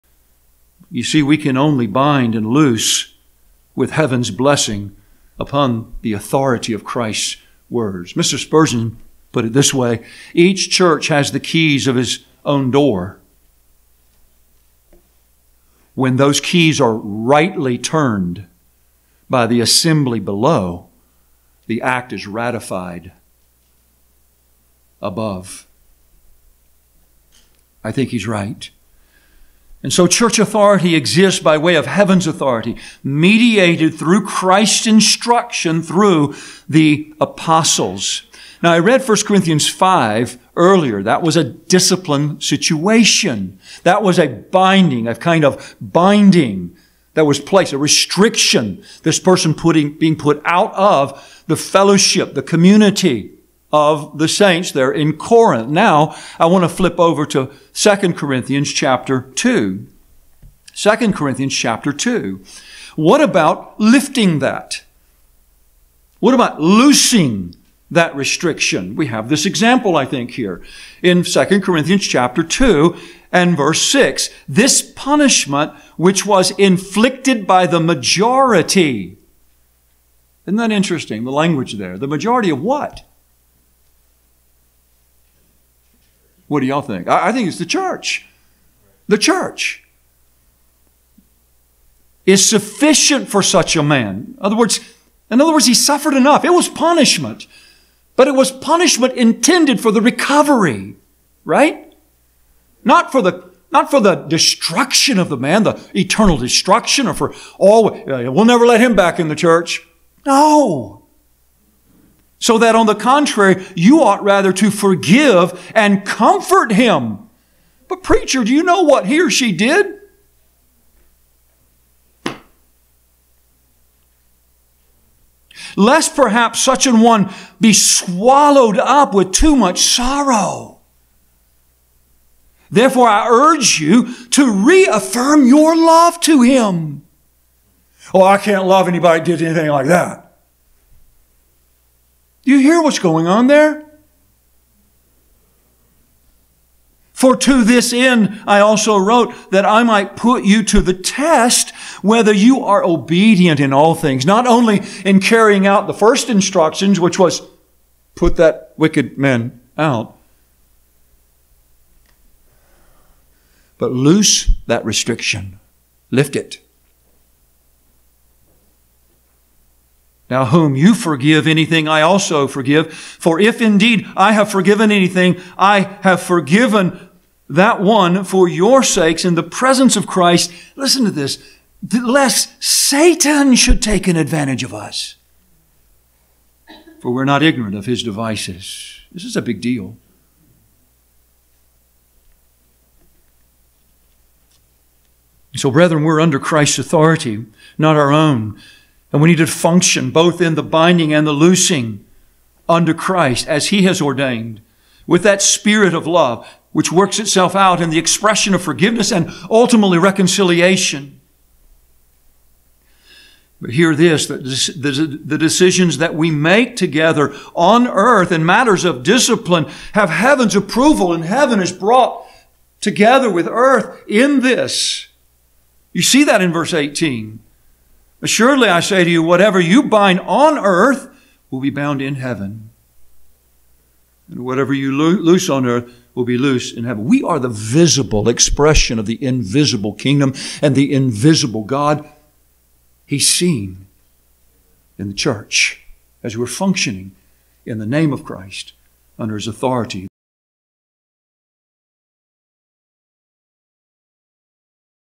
Loosing Equals Reconciliation
An excerpt from a message entitled, "The Church That Disciplines", given at our local assembly on January 14th, 2024.